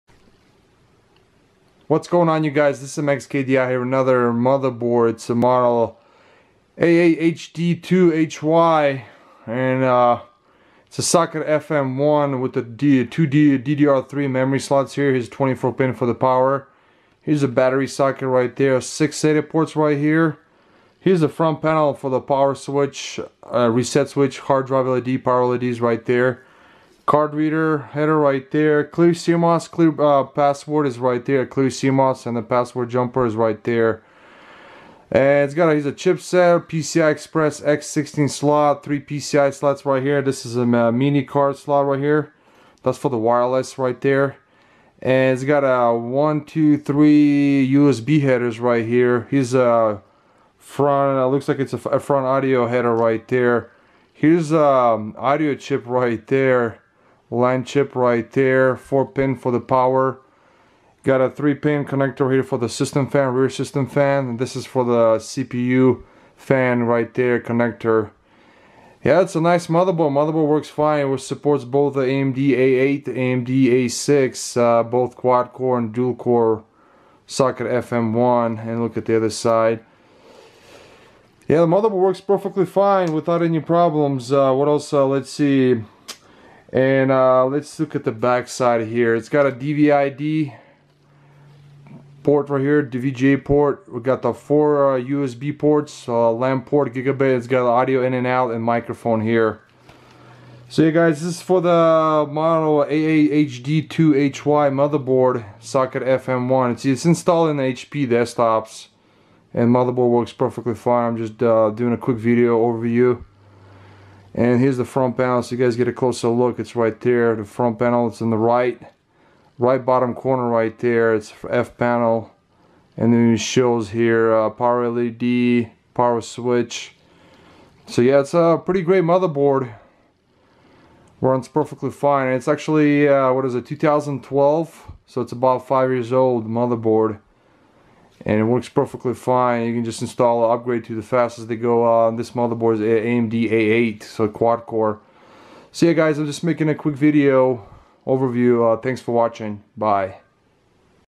AAHD2-HY Motherboard
This is an overview of the AAHD2-HY rev 1.03 Motherboard.

AMD Supported CPU's
Socket FM1 AMD  (Max 100w)
– A8-3850, A8-3800,
– A6-3650,  A6-3600,
– A4-3400, E2-3200